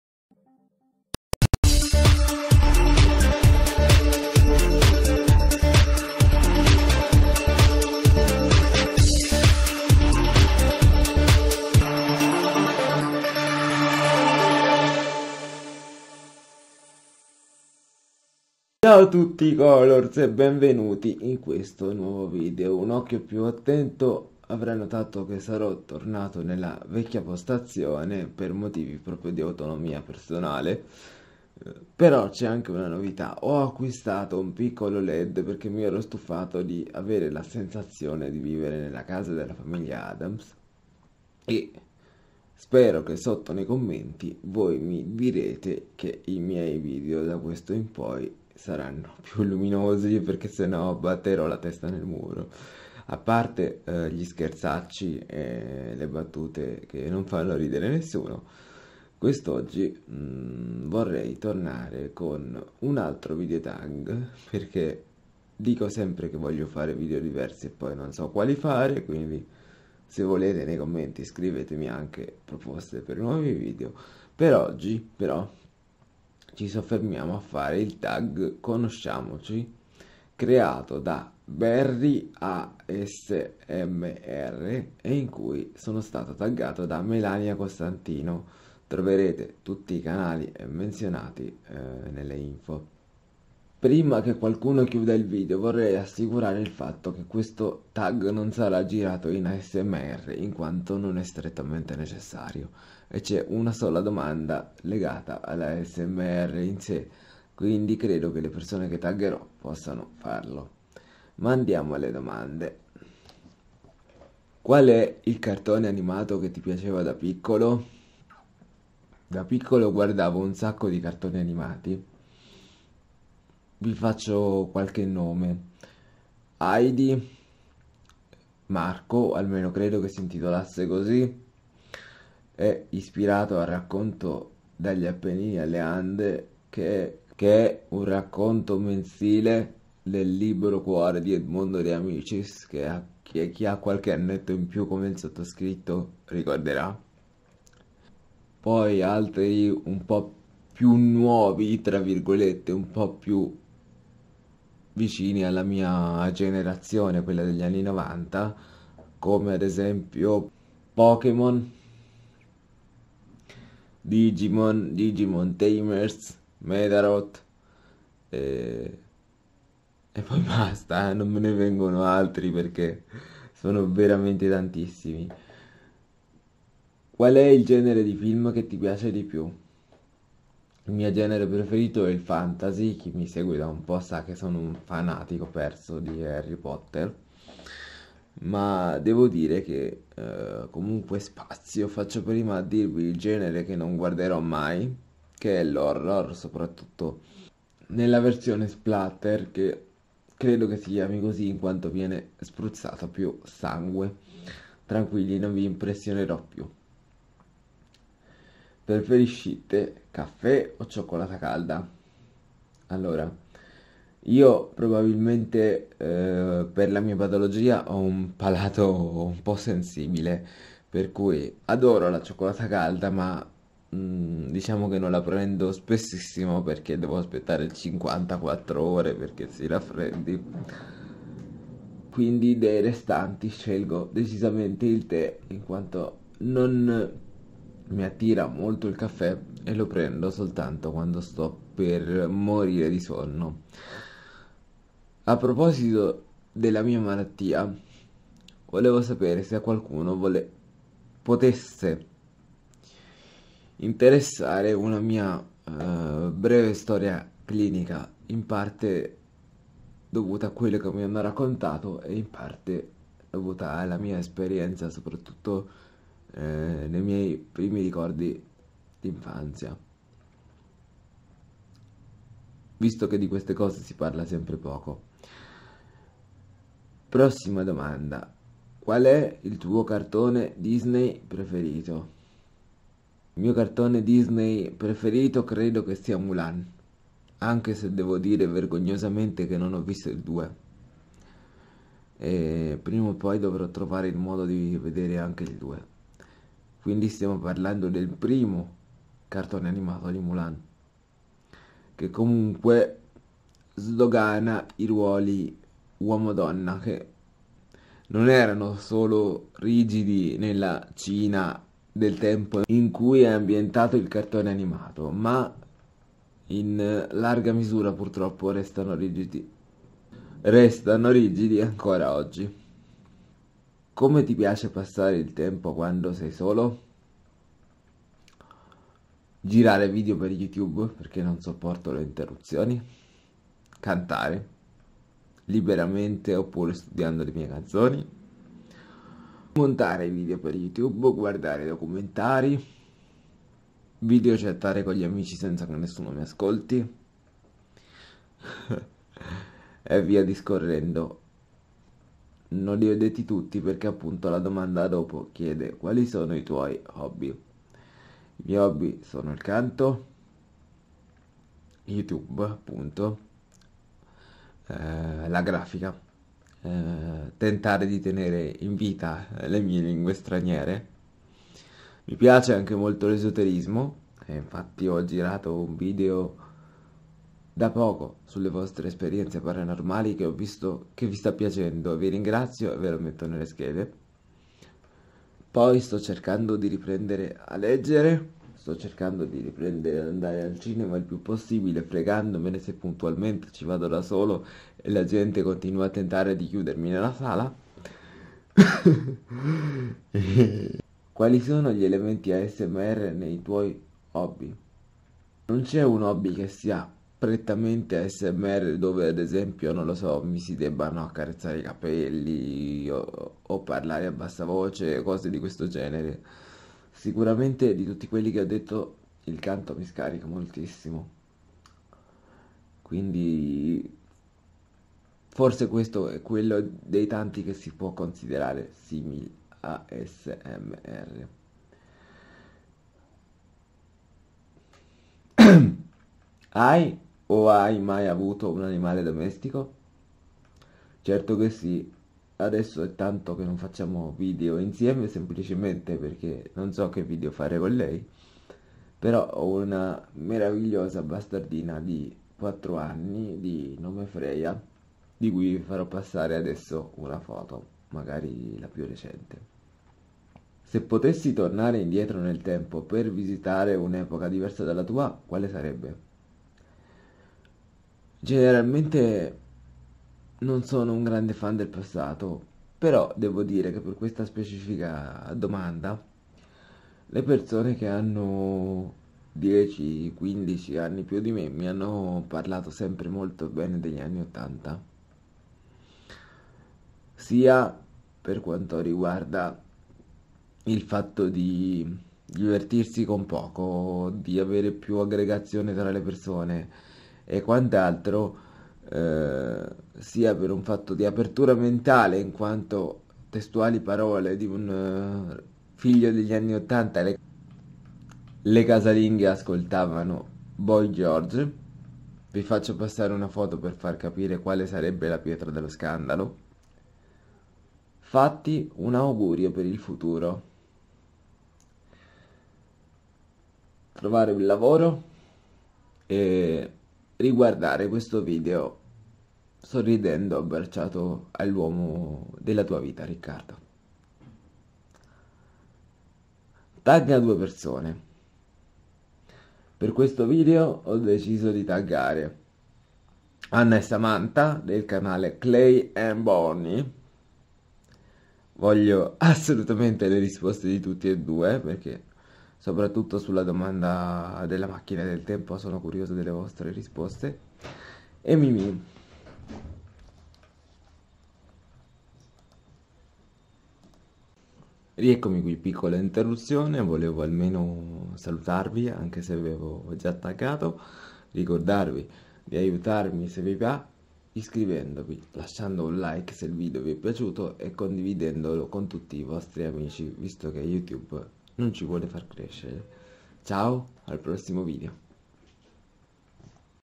CONOSCIAMOCI di Berry ASMR (video NO ASMR)
Ritorno ancora con un tag, perchè non so cosa postare!  Se vi va potete farmi richieste per  nuovi video, qui sotto nei commenti. Fatemi sapere anche se vi interessa il video che ho proposto!

DOMANDE

Qual è il cartone animato che ti piaceva da piccolo?
Qual è il genere di film che ti piace di più?
Preferisci tè, caffè o cioccolata calda?
Qual è il tuo cartone della Disney preferito?
Come ti piace passare il tempo quando sei solo?
Quali sono i tuoi hobby?
Quali sono gli elementi ASMR nei tuoi hobby?
Hai o hai mai avuto un animale domestico?
Se potessi tornare indietro nel tempo per visitare un’epoca diversa dalla tua, quale sarebbe?
Fatti un augurio per il futuro.
Tagga due persone.